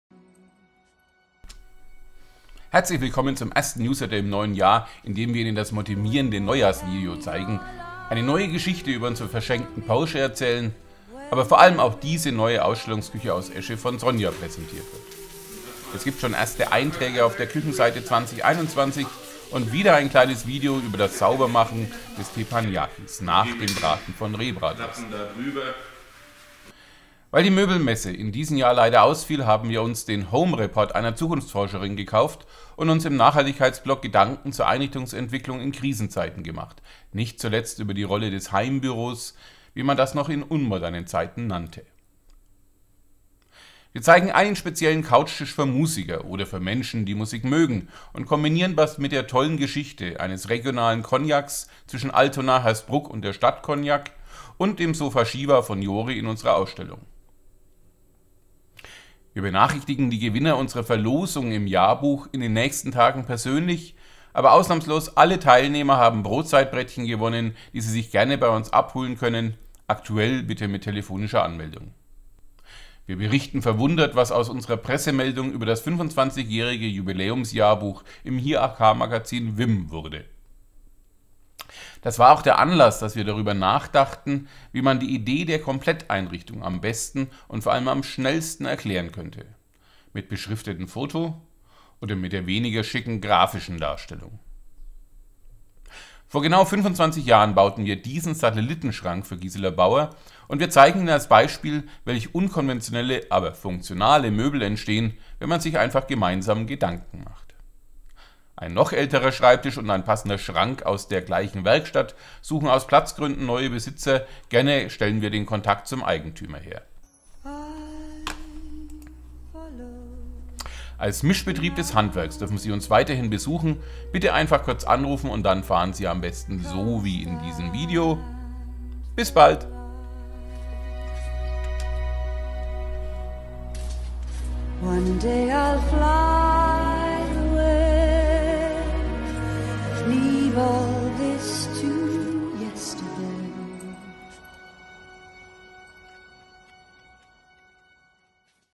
NL173 im Februar
Newsletter 173: Eschenküche mit Home-Office-Ausstattung, Bettwarenaktion, Tepan putzen, Kompletteinrichtung, Porsche wirkt, Musiker-Couchtisch, Coganc, Wimmelbild-Verlosung
Das haben wir für Sie gesammelt:
1. Verschenkter Porsche bringt Kritik und Kontakte
2. Neue Küche mit Hoffice-Steckdose u.v.m.
3. Zukunftsforschung zur Einrichtung statt Möbelmesse
4. Musiker-Couchtisch und regionaler Cognac
5. Jahrbuch-Preis-Verlosung und Pressemeldung
6. Satelittenschrank hat sich gut gehalten
7. Wie kann man Kompletteinrichtung abbilden?
8. Kurznachrichten +++ Pro Natura Sonderaktion Bettwaren + Edler Möbelmacher-Schreibtisch und -Schrank suchen neue Besitzer+